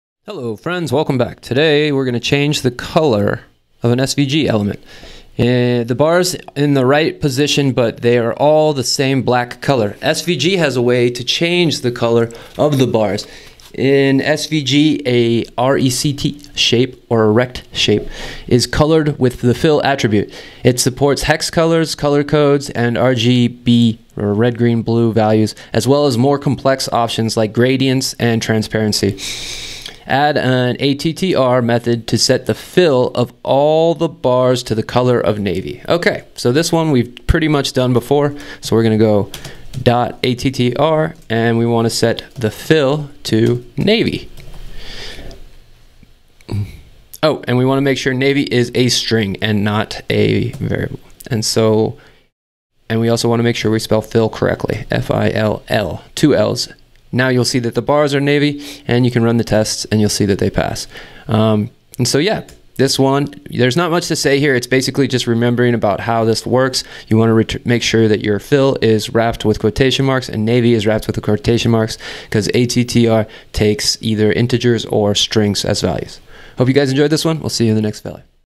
Change the Color of an SVG Element - Data Visualization with D3 - Free Code Camp
In this data visualization with D3 tutorial we change the color of an SVG element. This video constitutes one part of many where I cover the FreeCodeCamp curriculum. My goal with these videos is to support early stage programmers to learn more quickly and understand the coursework more deeply. Enjoy!